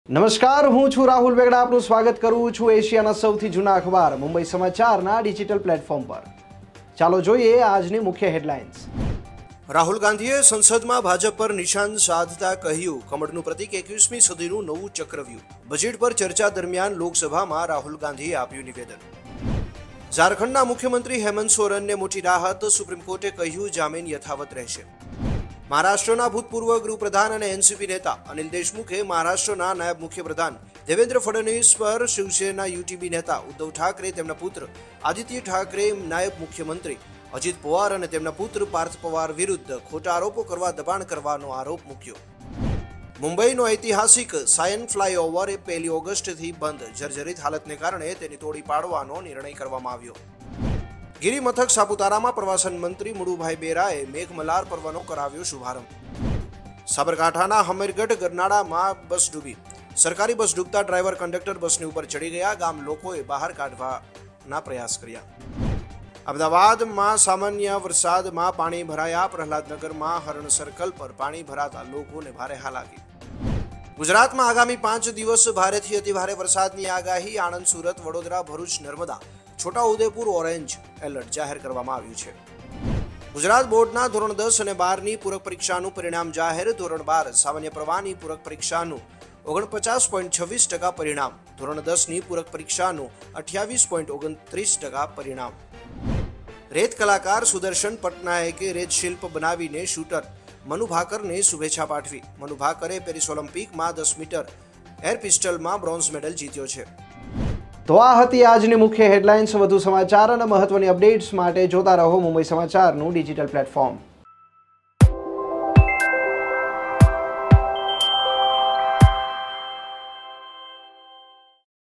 Today Top Afternoon News | 29 July | મુંબઇનો ઐતિહાસિક સાયન ફ્લાયઓવર 1 ઓગસ્ટથી બંધ | Mumbai Samachar
Today Top Afternoon News | 29 July | Mumbai's historic Sion flyover closed from August 1 Mumbai Samachar